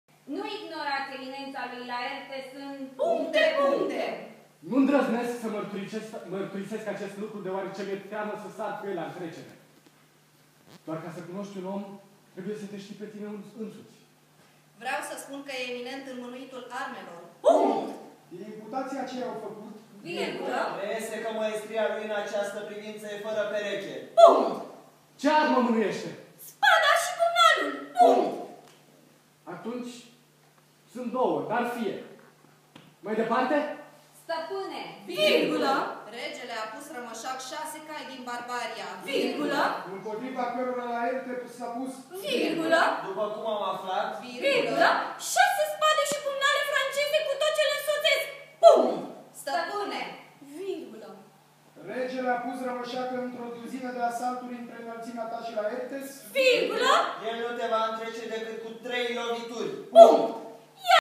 Hamlet... de hartie (27/29)
'Hamlet'
Departamentul de Arta Teatrala a Facultatii de Litere din cadrul Universitatii din Craiova
Regia Alex Teodorescu
Yannic Baecker,(Cluj) = Hamlet, Ionut Banica = Claudius, Theodora Balan = Gertrude, Petri Stefanescu = fantoma, Ellis Dinculeana = Polonius, Arina Pavalan = Ofelia, Danut Lihoaca = Laertes, Mihai Sirbu = Horatio, Laura Dumitrescu si Alina Paun = Rosencrantz si Guildenstern Victoria Zebunco = Moartea
22 decembrie 2018